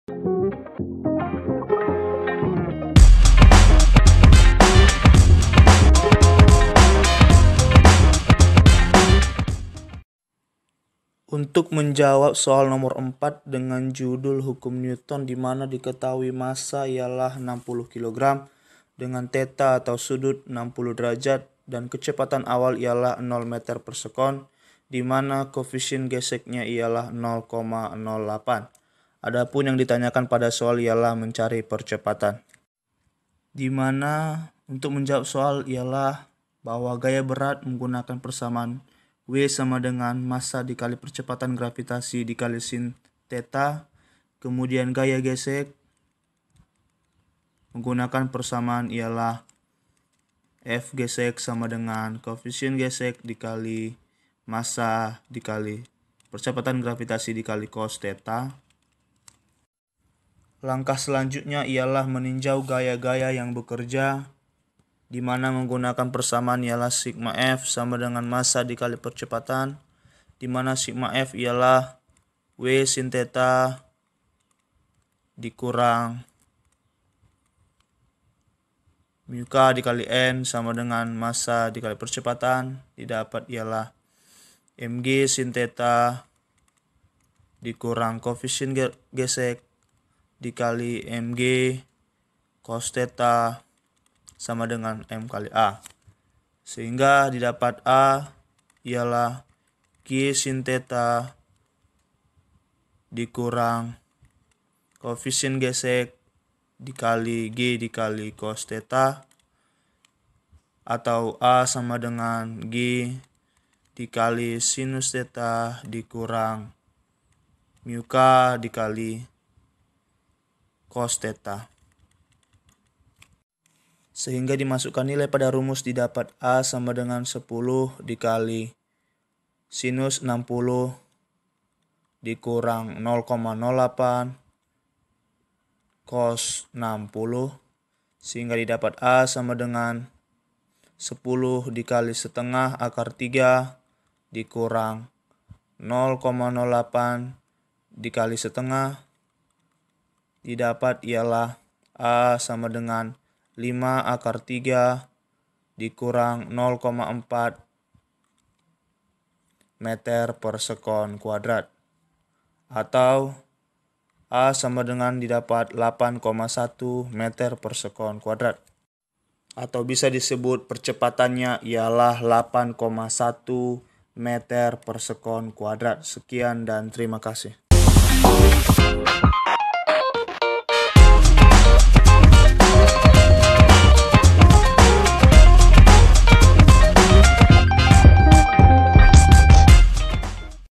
4. Seorang pemain ski bermassa 60 kg, melu... | Fisika SMA/MA K | Kelas 10 | 5
✅ Dalam video ini kita akan membahas: 4. Seorang pemain ski bermassa 60 kg, meluncur pada sebuah lereng dengan kemiringan 60°  tanpa kecepatan awal.
Jika diketahui koefisien gesekan salju dengan papan ski adalah 0,08, berapakah percepatan yang dialami orang tersebut?
Tema:
Kelas: 10
Mata Pelajaran: Fisika
Bab: 5
Buku: Fisika SMA/MA Kelas X

👉 Buku yang telah tersedia:
{PLAYLIST_LINK}
Di mana kamu dapat menemukan kami?